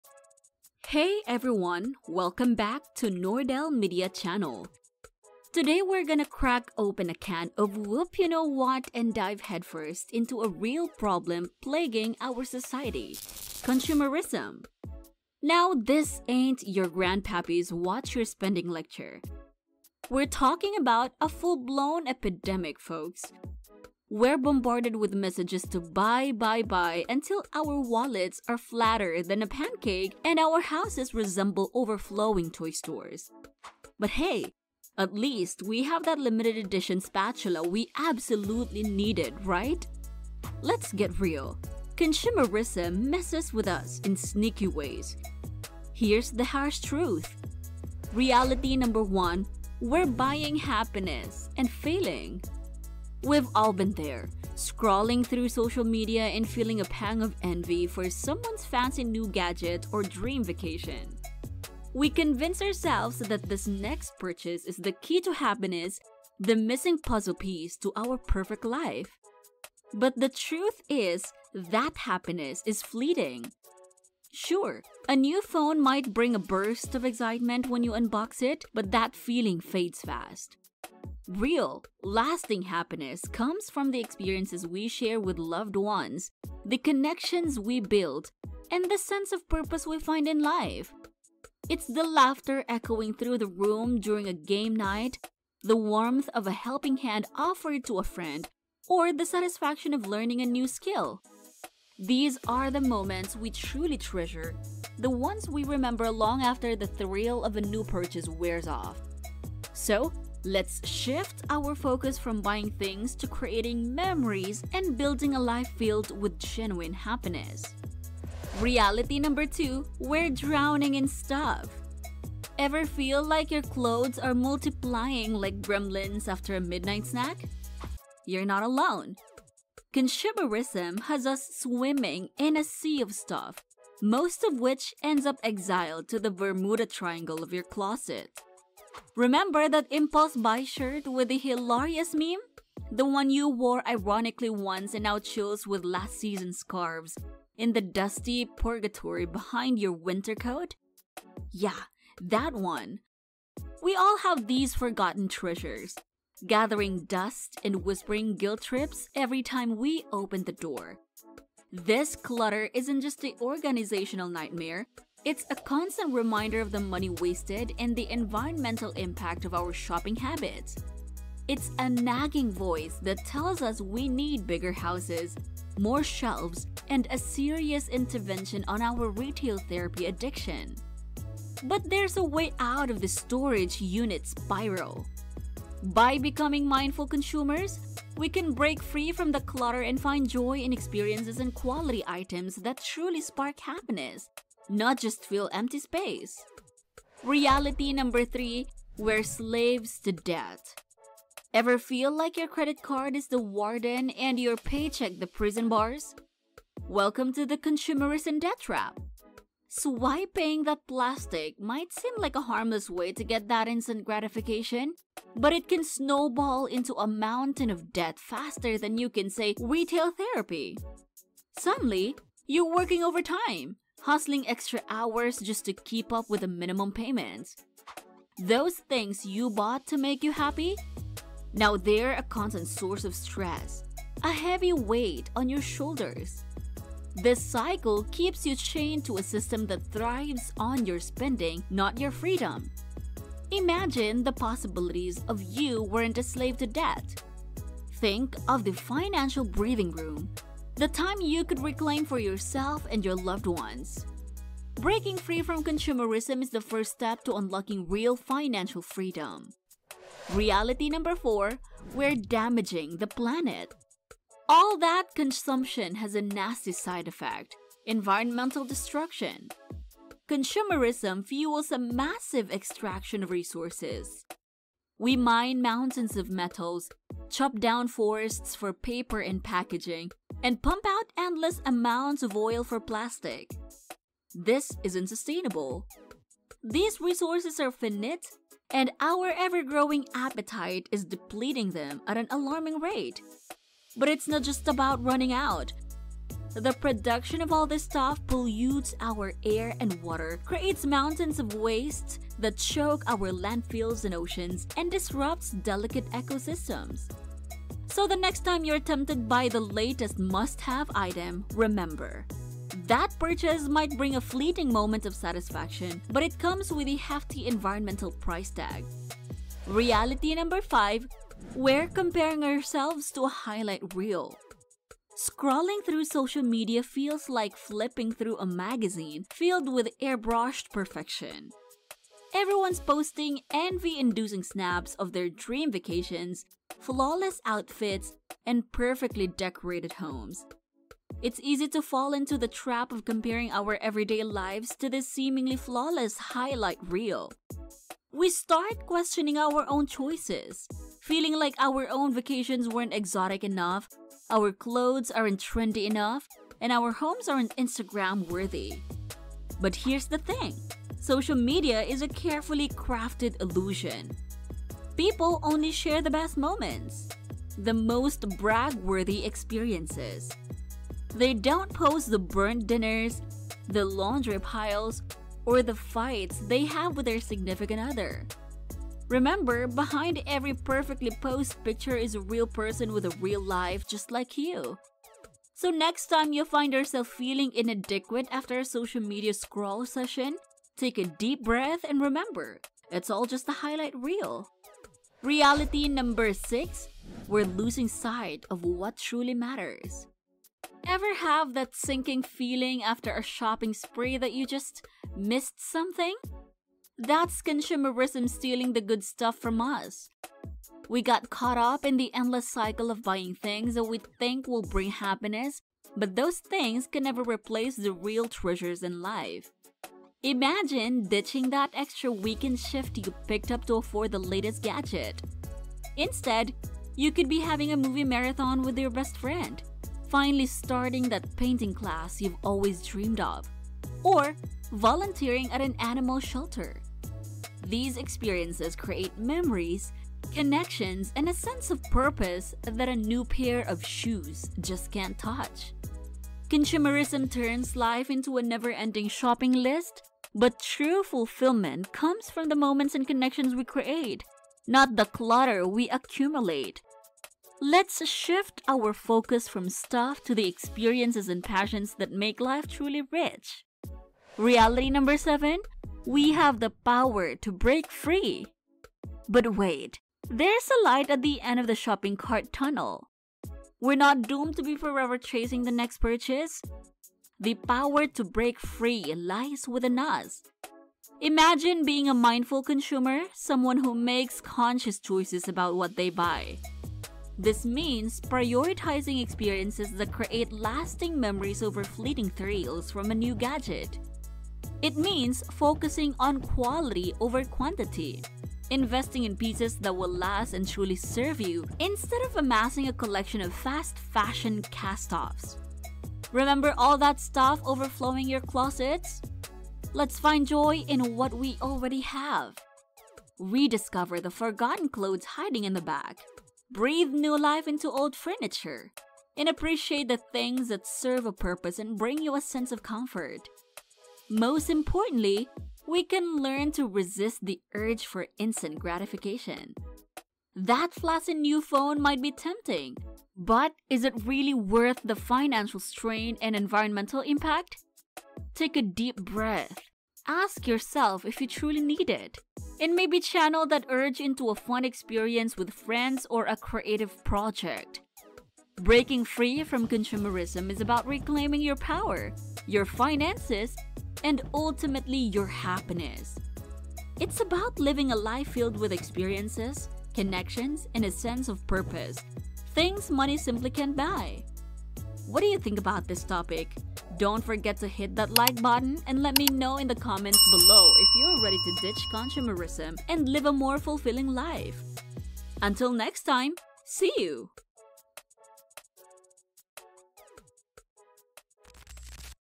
An Alarming Epidemic in America – Escape Consumerism NOW!
Are you trapped in the cycle of relentless spending and never-ending consumption? In this eye-opening video, we delve into the alarming consumerism epidemic sweeping across America. Discover how this culture of constant buying is not only draining your wallet but also impacting your mental health and overall happiness. We'll expose the hidden forces driving this behavior and offer actionable tips to help you break free. Learn how to recognize the signs of consumerism, make mindful financial choices, and reclaim control over your life. By escaping this trap, you can achieve true financial freedom and find lasting contentment beyond material possessions. Don't let consumerism dictate your life – watch now and start your journey towards a more fulfilling, financially secure future.

Please follow in Instragram: nordelmedia